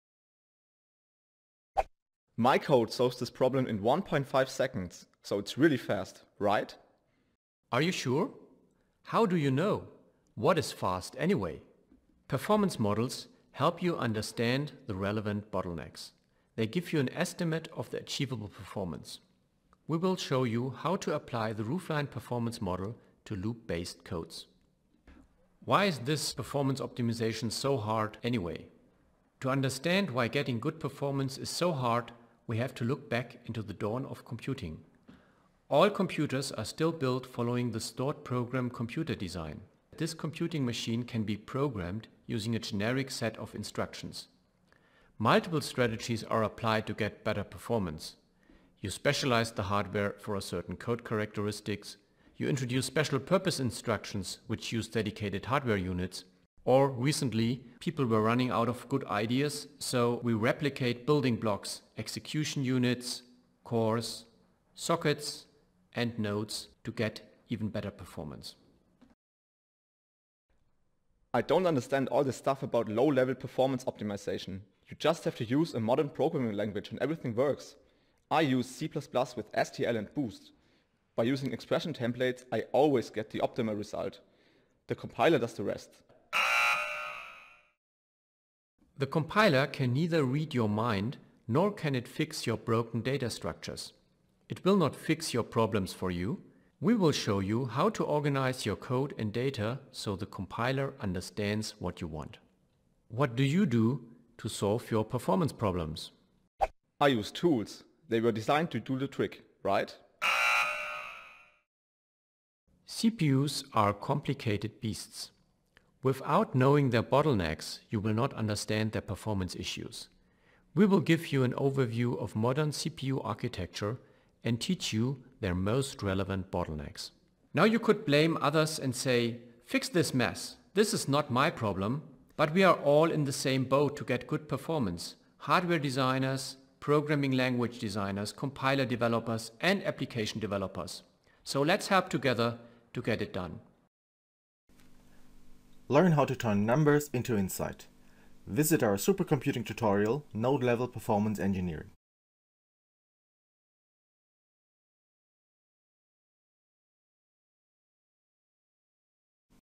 SC18 Tutorial "Node-Level Performance Engineering"
The Kay Bailey Hutchison Convention Center, Dallas, TX, Sunday, November 11, 2018, 8:30am-5pm.

Learn how to turn numbers into insight: Our SC18 tutorial shows you the basics of node-level computer architecture and performance modeling.

Code optimization is complicated. Often developers blindly try textbook optimizations and hope their code will run faster. It is far more effective to know beforehand how fast the code can possibly run. In this tutorial we teach the architectural features of current processor chips, multiprocessor nodes, and accelerators, as far as they are relevant for the practitioner. SIMD vectorization, shared vs. separate caches, bandwidth bottlenecks, hyperthreading, and ccNUMA characteristics are introduced, and the influence of system topology and affinity on the performance of typical parallel programming constructs is demonstrated. The goal is to understand the performance bottlenecks at hand and to assess the impact of possible code optimizations. A cornerstone of these concepts is the roofline model, which is described in detail, including useful case studies and limits of its applicability.